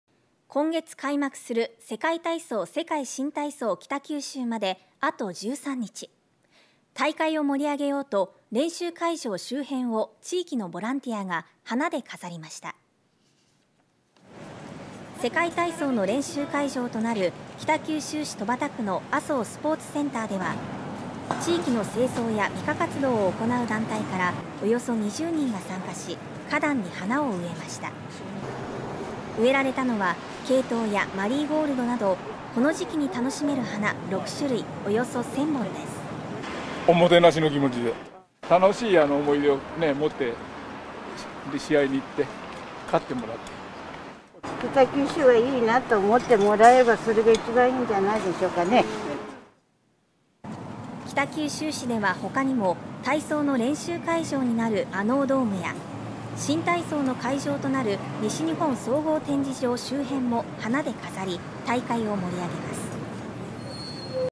「世界体操 世界新体操 北九州」練習会場を花で飾る
「世界体操世界新体操北九州」の開幕まであと１３日。大会を盛り上げようと練習会場周辺を地域のボランティアが花で飾りました。

世界体操の練習会場となる北九州市戸畑区の「浅生スポーツセンター」では、地域の清掃や美化活動を行う団体からおよそ２０人が参加し花壇に花を植えました。

植えられたのはケイトウやマリーゴールドなどこの時期に楽しめる花６種類およそ１０００本です。

北九州市では他にも体操の練習会場になる穴生ドームや新体操の会場となる西日本総合展示場周辺も花で飾り大会を盛り上げる予定で参加した女性は「北九州市いいなぁ、と思ってもらえれば」と話していました。